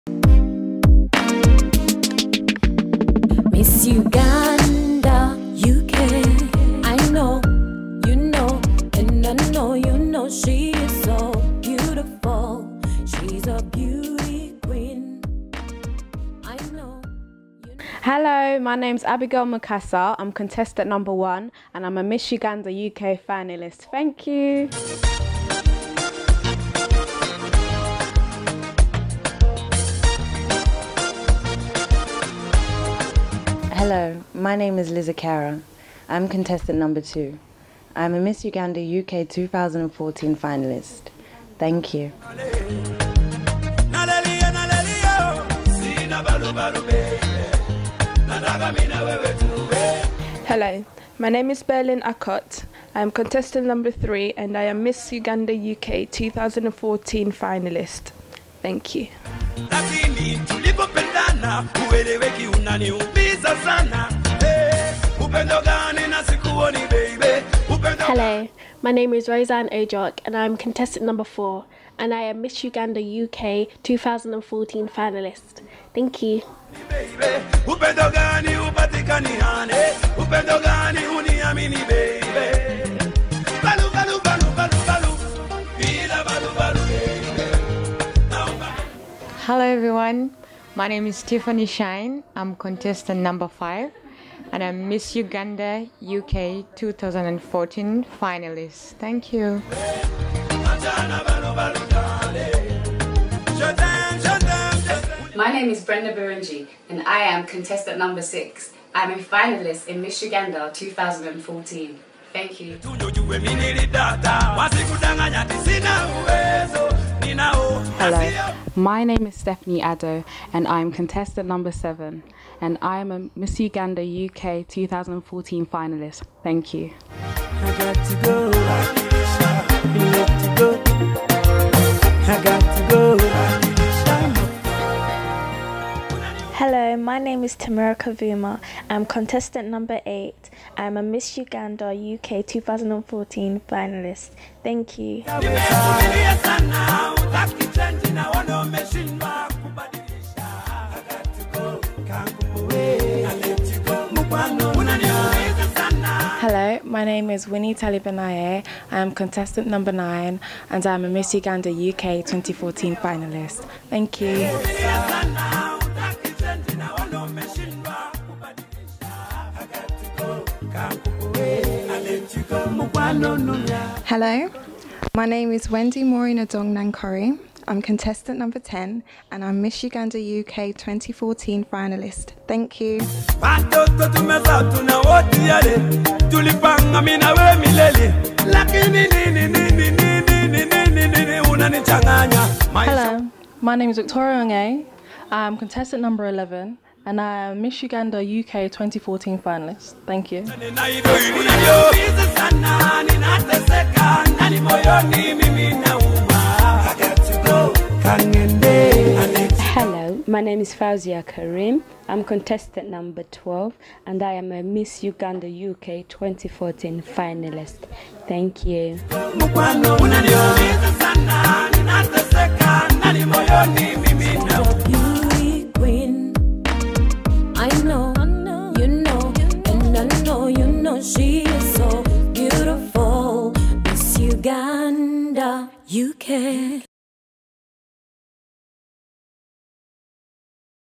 [Official] Miss Uganda UK 2014 Finalists
Meet Miss Uganda UK 2014 Contestants
Finale on September 20th 2014
At Stratford Town Hall
Find us on Istagram..Facebook and Twitter @MissUgandaUK